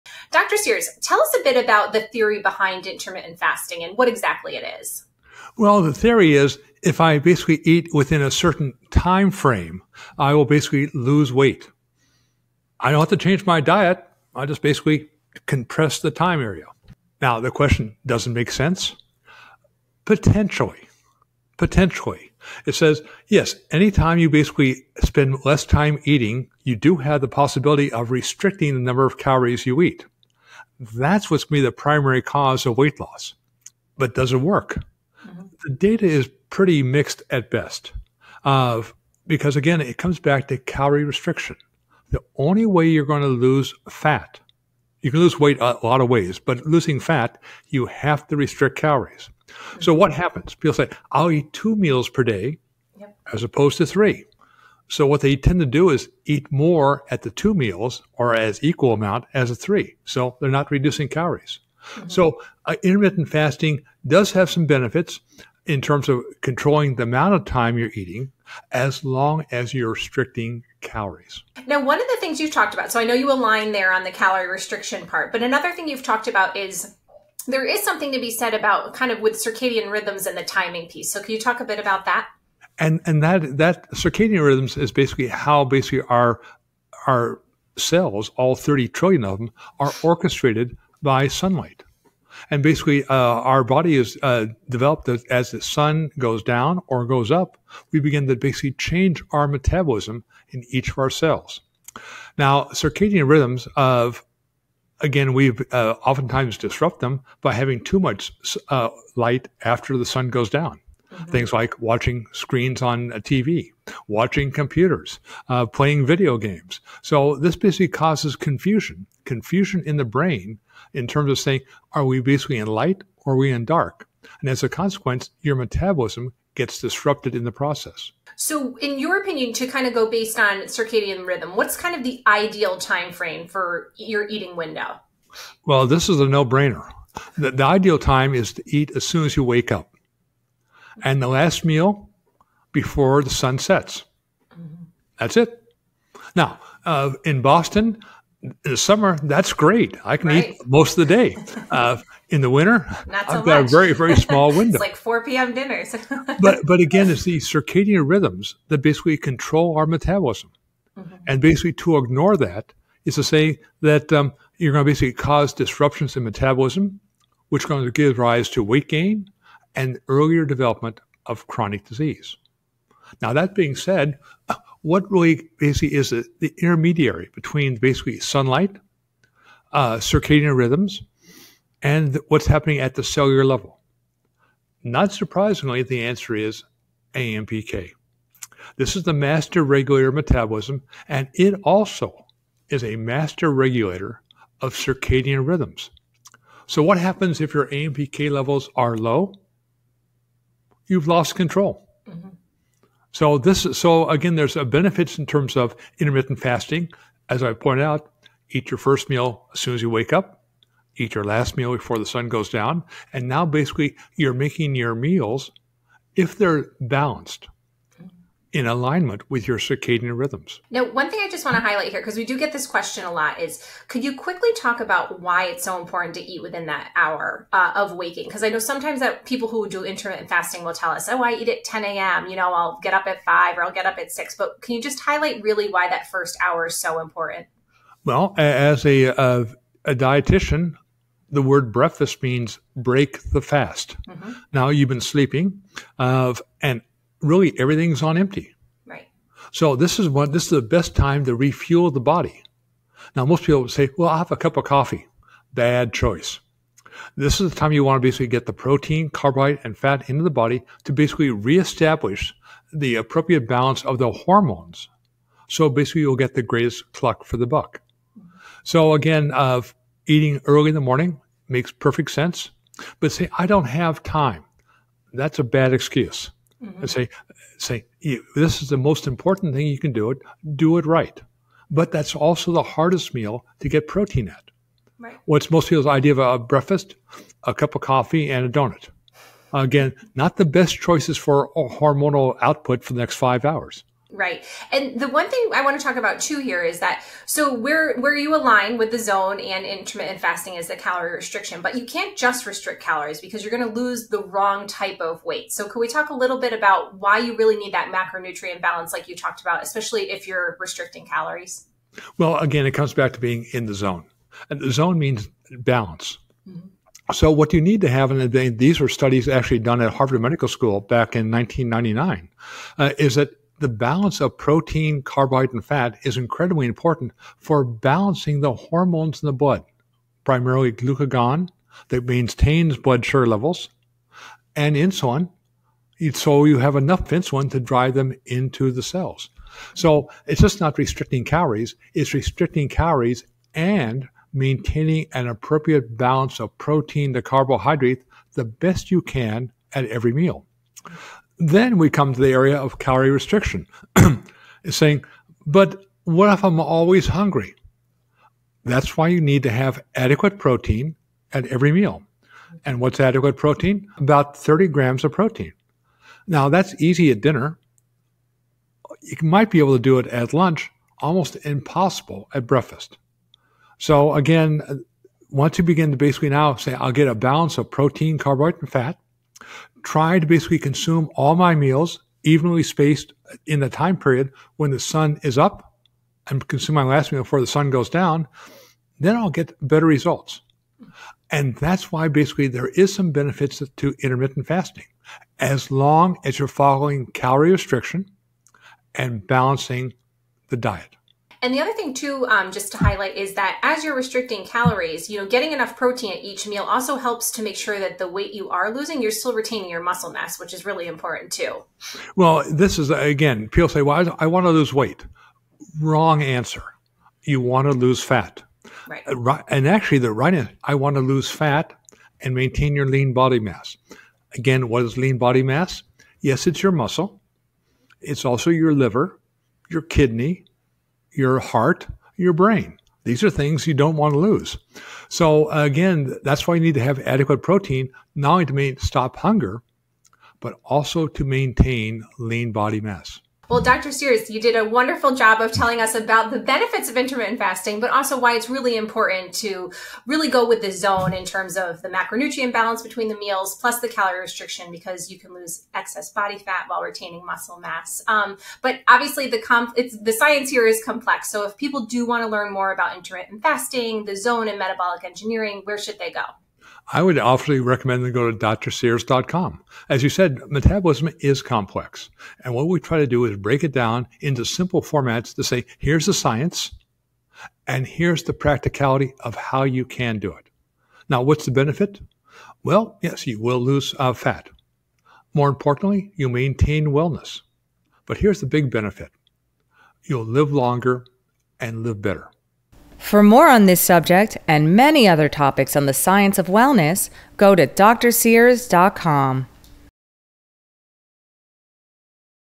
The Facts About Intermittent Fasting
Welcome to The Wellness Zone Podcast with Dr. Barry Sears and Mary Perry. This week's topic: Intermittent Fasting.

00:00  Theory behind intermittent fasting
01:23 Eating to circadian rhythms
02:15 When you should eat if doing intermittent fasting
05:12 Zone diet and calorie restriction
07:47 Learn more at DrSears.com